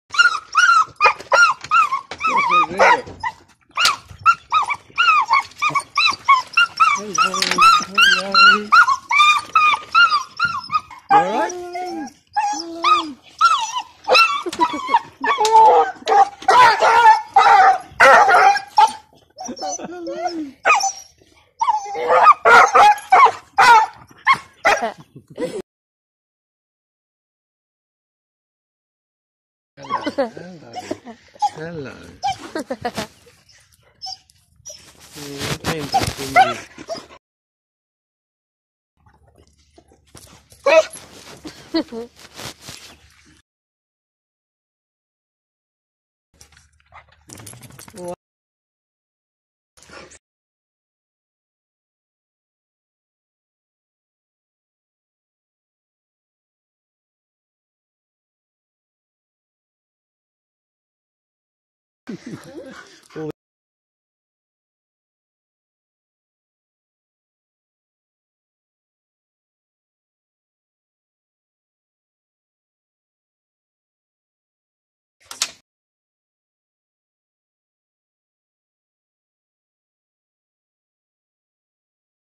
Beagle reunited with owner after 6 weeks away!
Cute beagle reunited with owner after 6 weeks away!